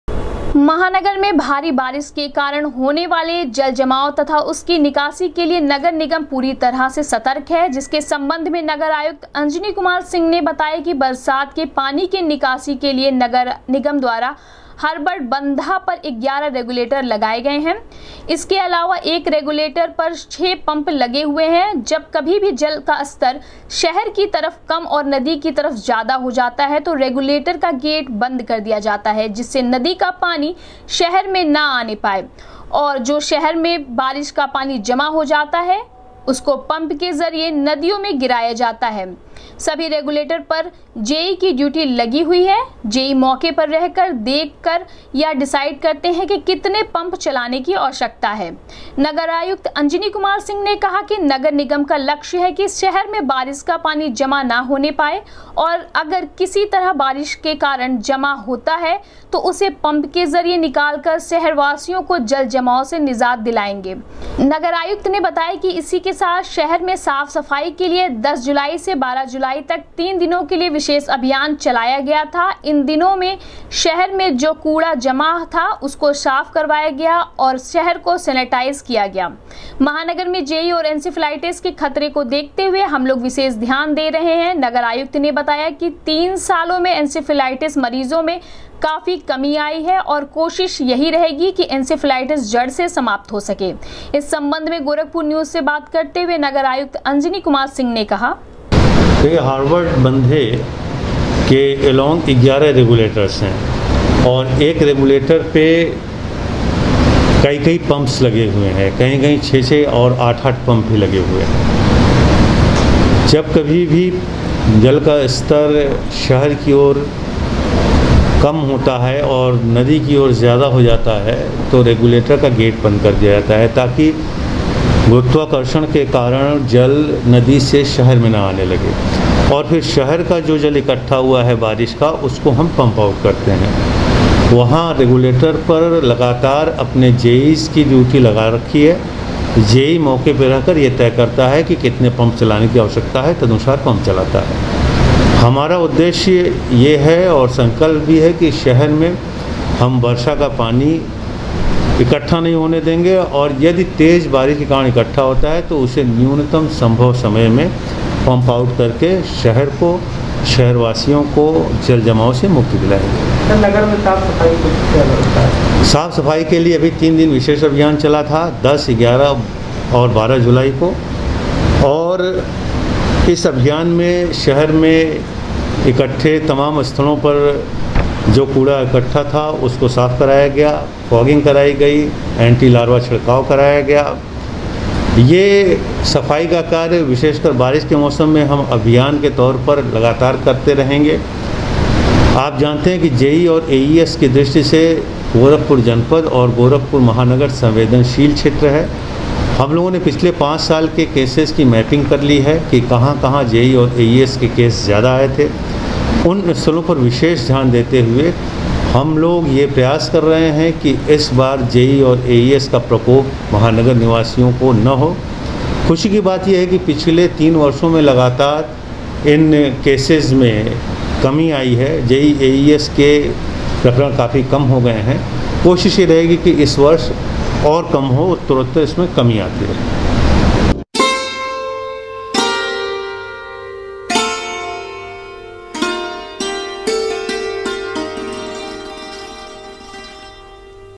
गोरखपुर न्यूज से बात करते हुए नगर आयुक्त ने कहा,महानगर को जलभराव से बचाने के लिए कार्य कर रहे है 11
गोरखपुर न्यूज से बात करते हुए नगर आयुक्त ने कहा,महानगर को जलभराव से बचाने के लिए कार्य कर रहे है 11 रेगुलेटर,साफ सफाई की भी है समुचित व्यवस्था जिससे मिल सके इंसेफेलाइटिस ने निजात